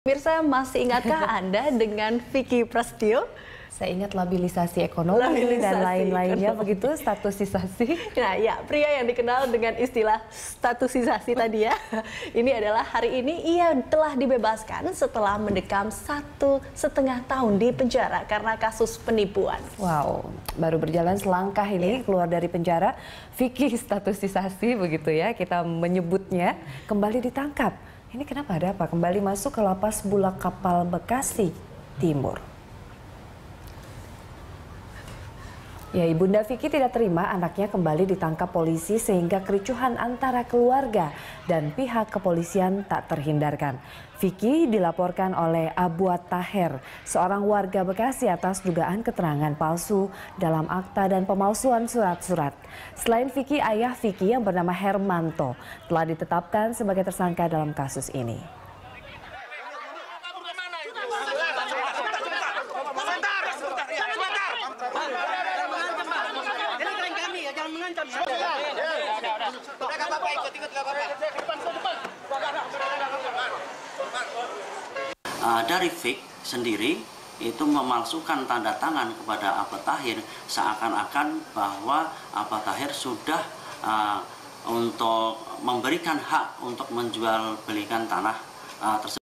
Baru Bebas, Vicky “Statutisasi” Prasetyo Ditahan Lagi
Vicky Prasetyo, pria yang dikenal dengan istilah statutisasi telah bebas , Sabtu (8/11/2014), usai mendekam di Lapas Bulak Kapal, Bekasi Timur, terkait kasus pemalsuan surat tanah.

Facebook.com/BeritaSatuTV
Youtube.com/BeritaSatu